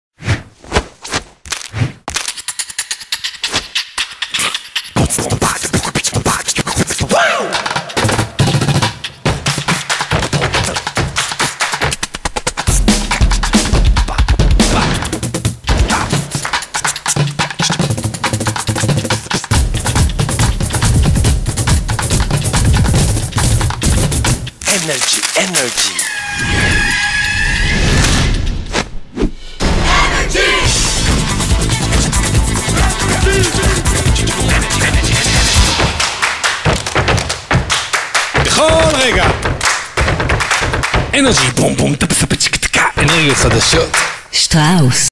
אנרג'י בכל רגע-להקת מיומנה מפציצה באנרגיות(שטראוס)
שיתוף הפעולה בין אנרג'י(שטראוס) ולהקת מיומנה בקמפיין חדש ומלא אנרגיות, ריקוד, תיפוף והרבה ווליום. תנסו לעמוד בקצב.